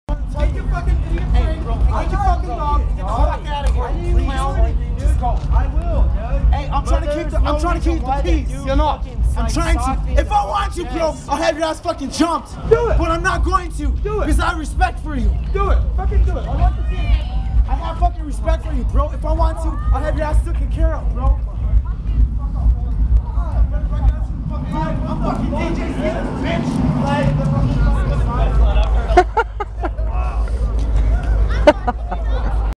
seattle drama behind a rave
Another Four No Reason Production. this is behind the king kat venue at a rave...mindless yelling and empty threats...lol